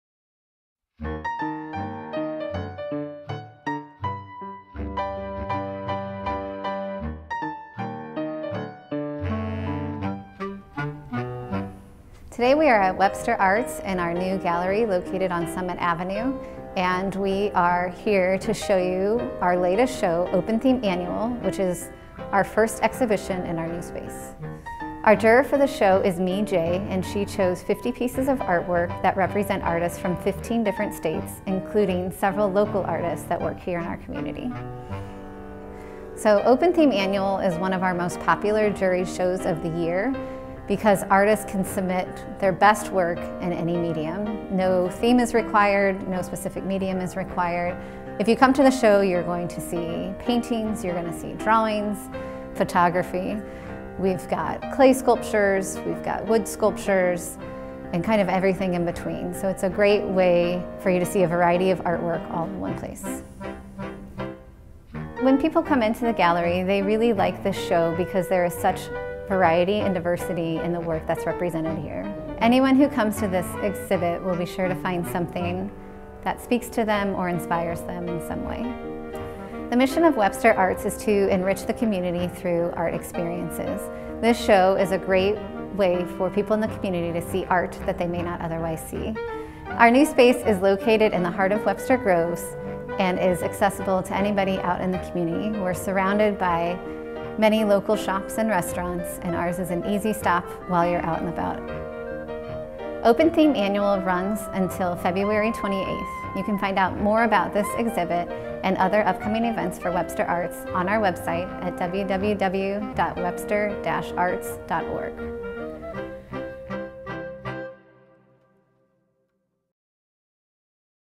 Webster Arts Open Theme Annual 2022
Webster Arts' Open Theme Annual features 50 different works of art from around the United States. As there were no restrictions on the pieces that could be submitted, this juried show is a menagerie of diverse mediums and ideas. This is also the first exhibit on show within Webster Arts' new gallery space on Summit Avenue, in the heart of Webster Groves, MO.
Open Theme Annual continues at Webster Arts through February 28, 2022.